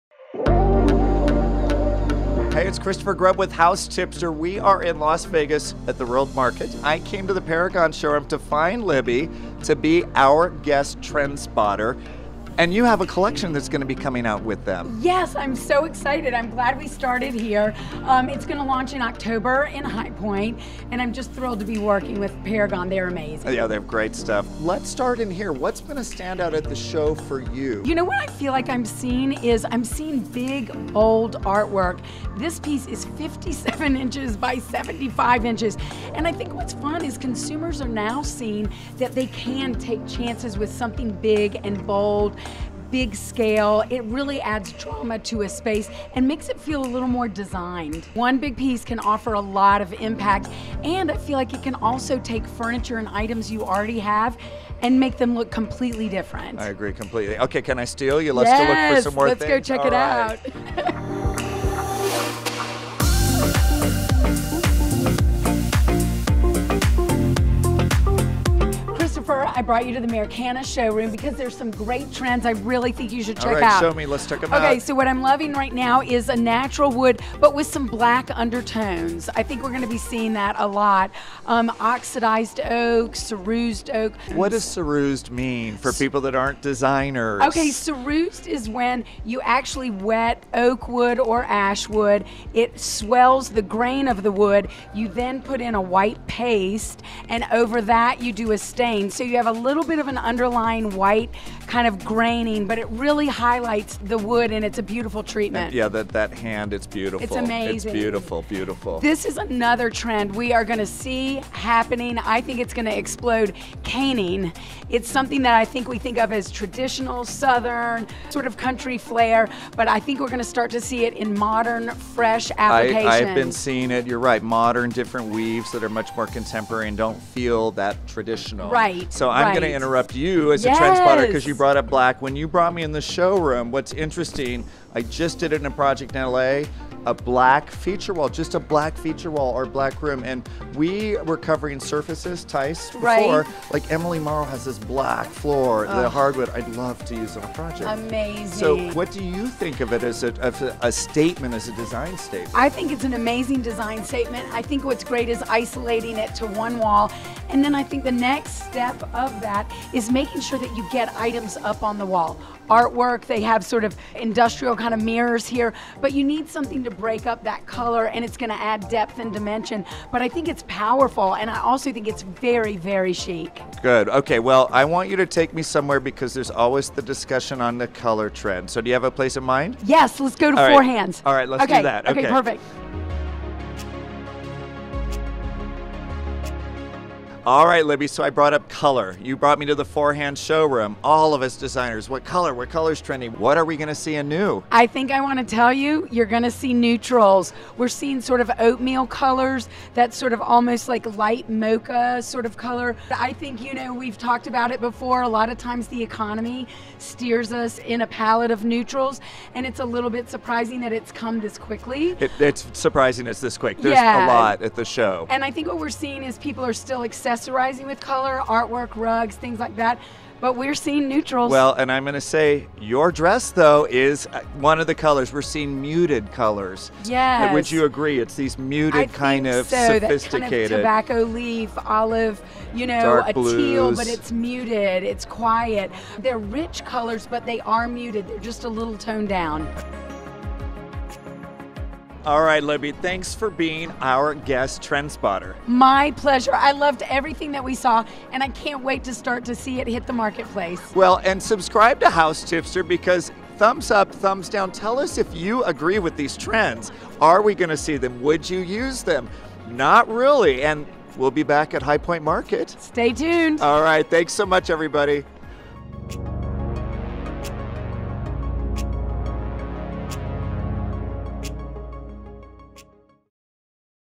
Trendspotting with Libby Langdon at Las Vegas Market 2019
Las Vegas Market is filled with so many incredible designers, vendors, and showcases, it can be hard to keep up with all the stunning innovations! That’s why Arch-Interiors designer and House Tipster host Christopher Grubb enlisted the help of interior designer Libby Langdon to break down some of the most important trends she sees emerging this Winter 2019 season. In this video, we follow the duo around a few of the best showrooms to get caught up with new artworks, design focal points, and more.